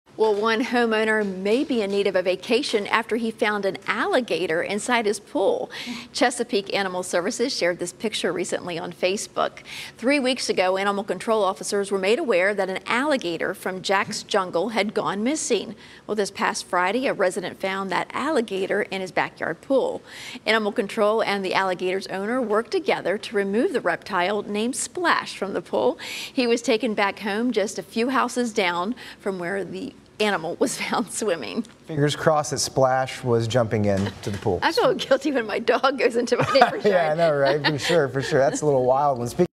Alligator rescued from pool in Virginia
n alligator is safe after being found swimming in a Chesapeake homeowner's pool.